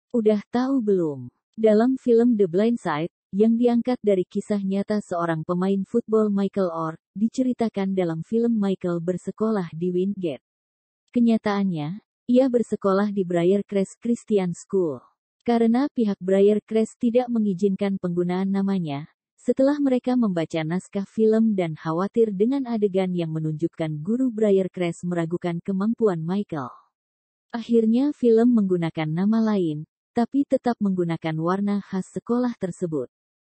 Dibalik The Blind Side
Sedikit fakta, cerita, berita kali ini dari film The Blind Side yang mungkin kamu belum tahu.
Yuk Simak